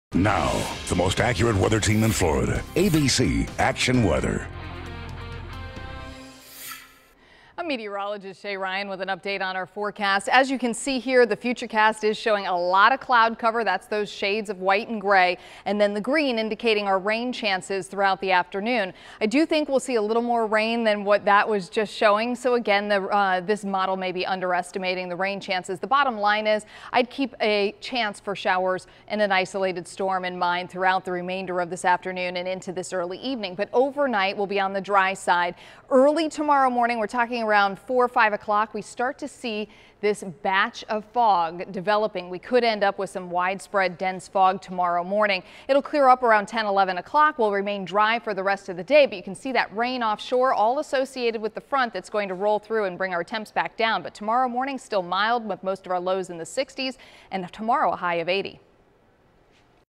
Florida's Most Accurate Forecast with Shay Ryan on Tuesday, November 21, 2017
Mostly cloudy with rain at times